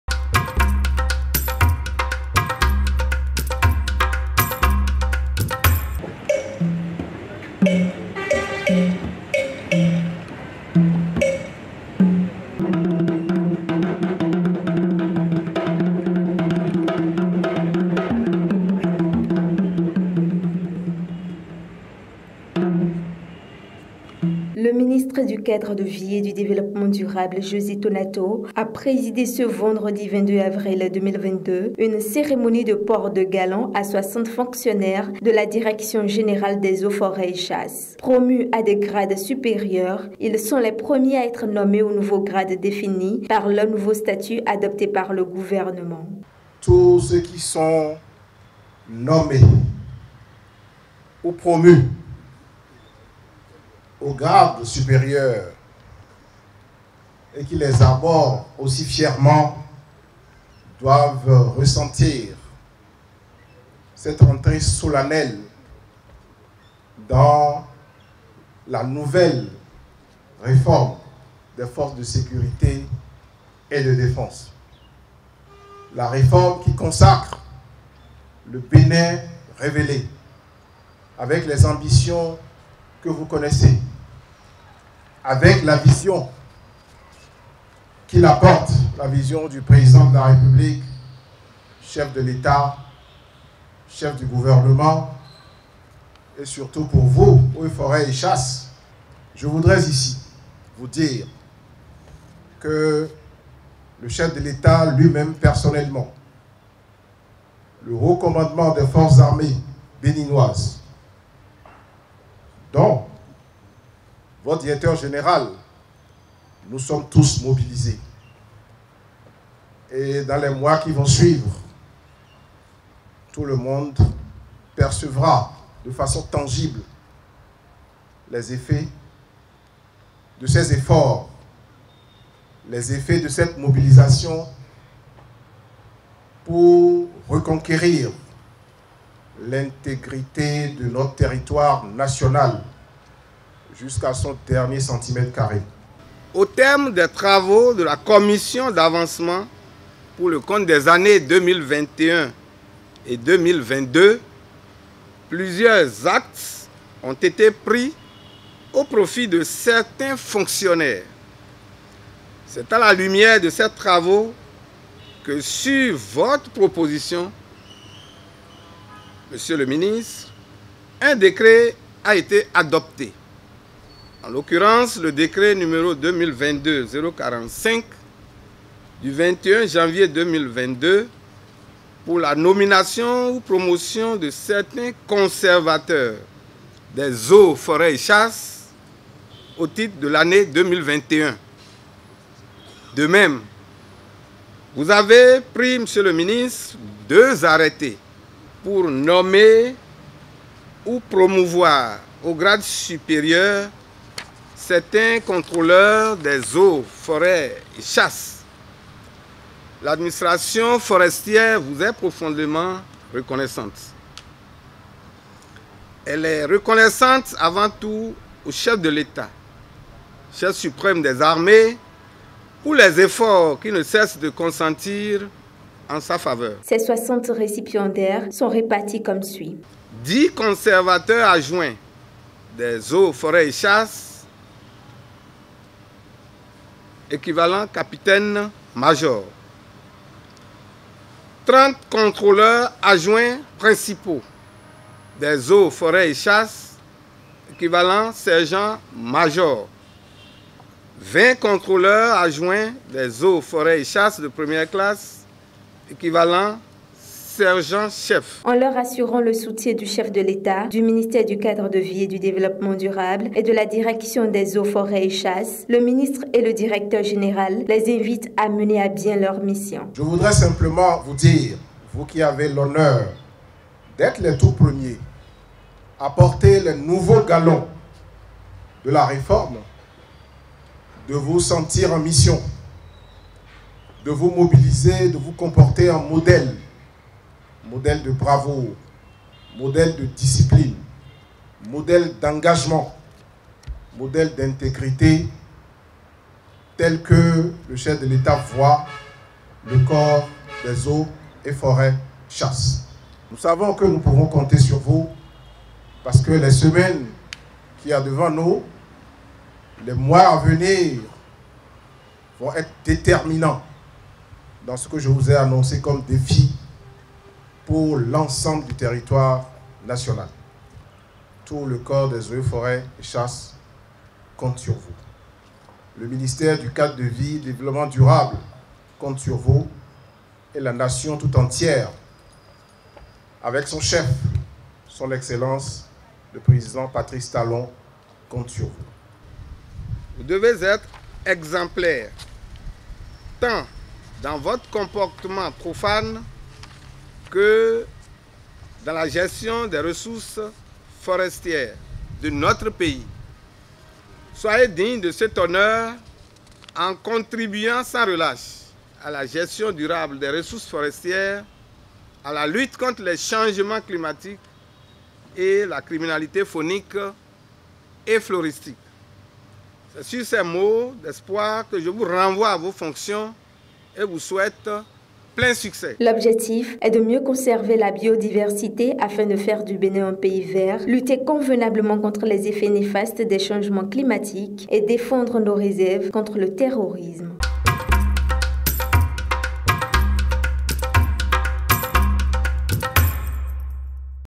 JOSÉ TONATO PORTE LE GALON À 60 OFFICIERS DES EAUX, FORÊTS ET CHASSE DU BÉNIN.
Le Ministre du cadre de vie et du développement durable José TONATO a présidé ce vendredi 22 avril 2022, une cérémonie de port de galons à 60 fonctionnaires de la Direction générale des eaux forêts et chasse. Promus à des grades supérieurs, ils sont les premiers à être nommées aux nouveaux grades définis par leurs nouveaux statuts adoptés par le gouvernement.

Dans cette vidéo, vous aurez le compte rendu de KINGOTVWORLD de JOSÉ TONATO PORTE LE GALON À 60 OFFICIERS DES EAUX, FORÊTS ET CHASSE DU BÉNIN.